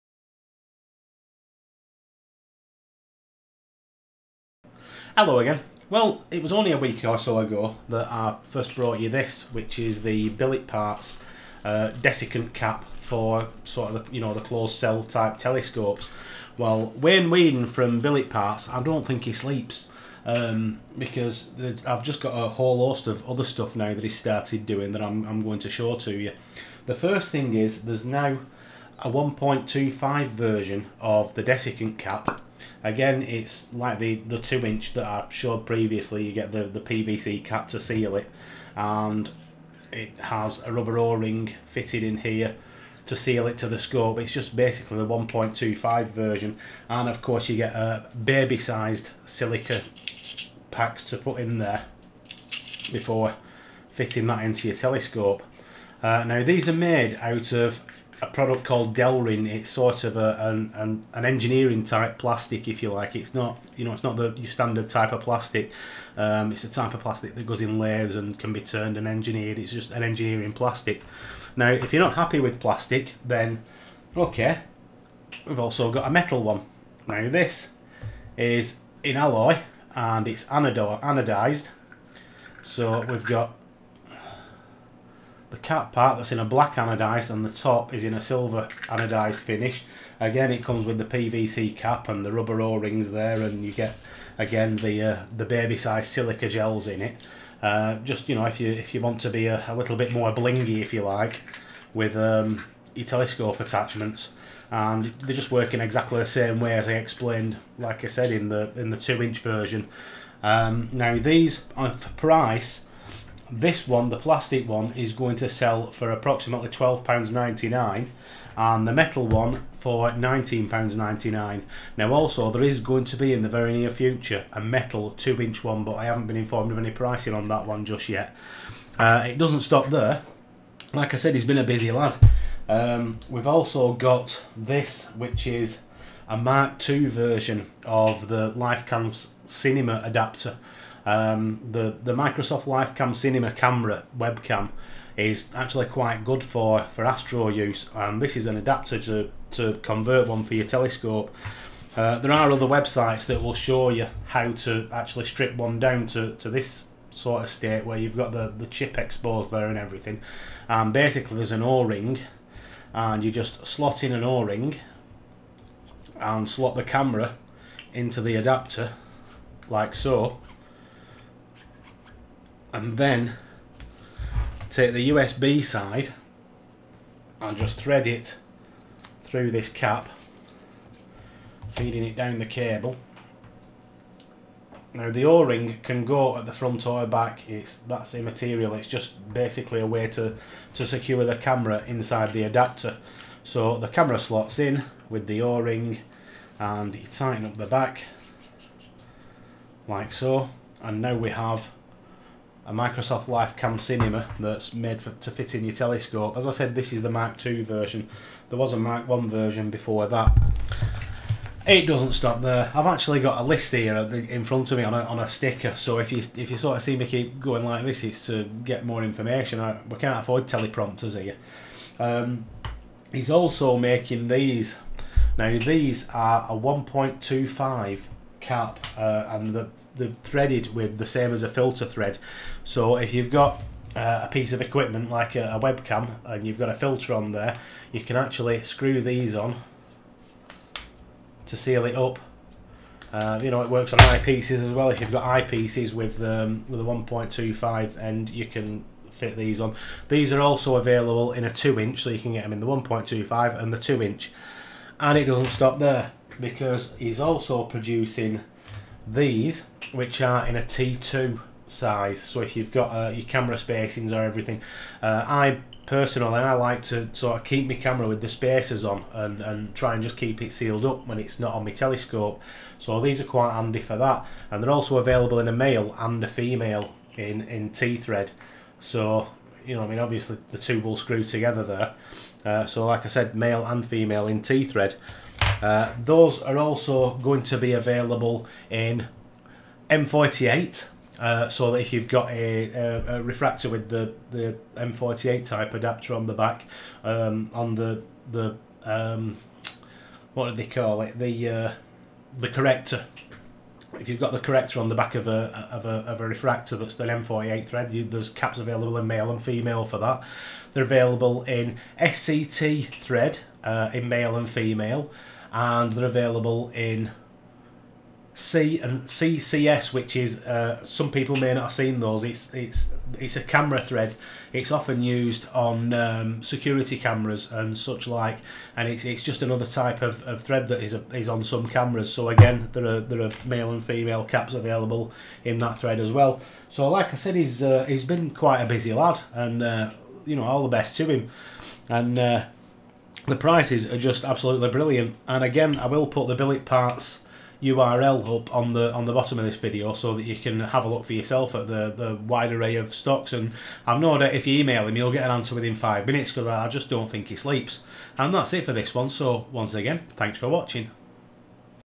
Billetparts Caps, Desiccant Caps & Adapters
A brief rundown on more astronomy equipment available from billetparts, including dust caps, desiccant caps and webcam adapters